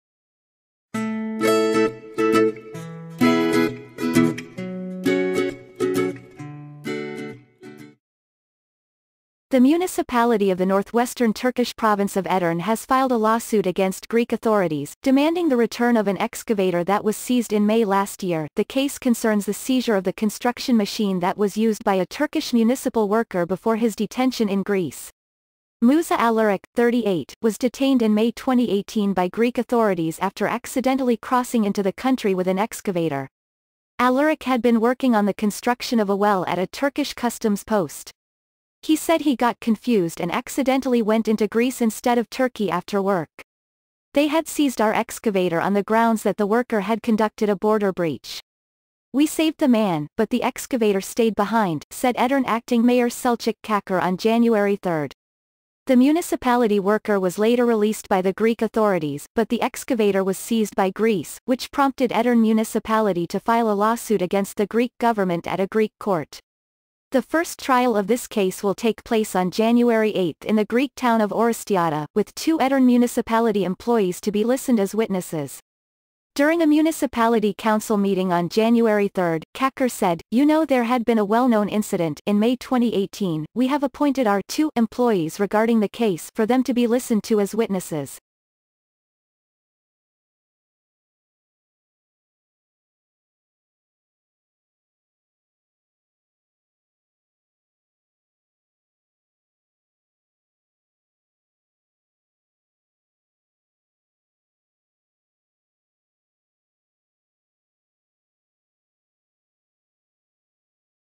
Edirne Municipality files lawsuit against Greece to get excavator back
The municipality of the northwestern Turkish province of Edirne has filed a lawsuit against Greek authorities, demanding the return of an excavator that was seized in May last year..